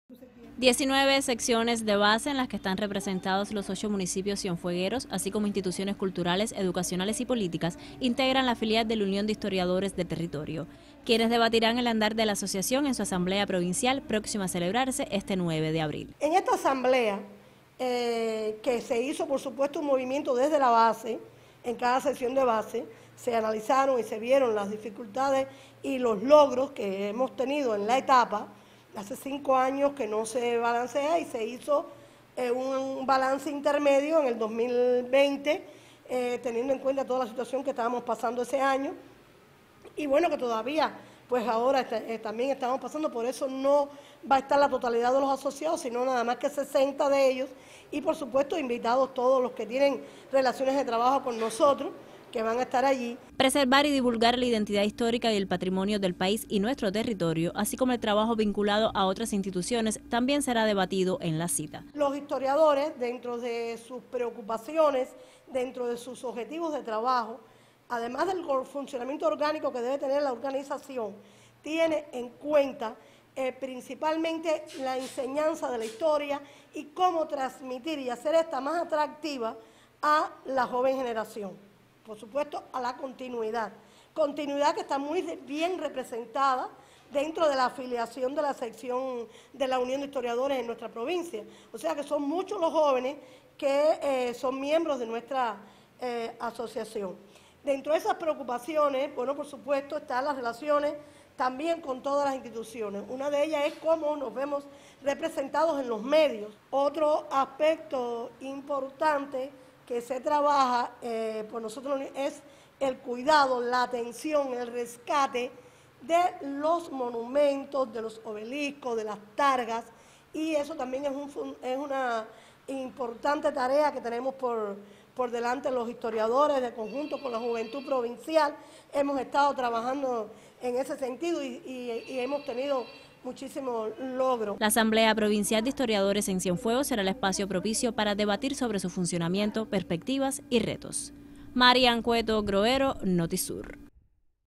Prepara asamblea de balance Filial de Unión de Historiadores de Cienfuegos
Integrantes de la Filial cienfueguera de la Unión de Historiadores realizarán próximamente, su asamblea de balance. En la cita debatirán sobre el funcionamiento y perspectivas de la organización, para fortalecer la memoria histórica local y nacional.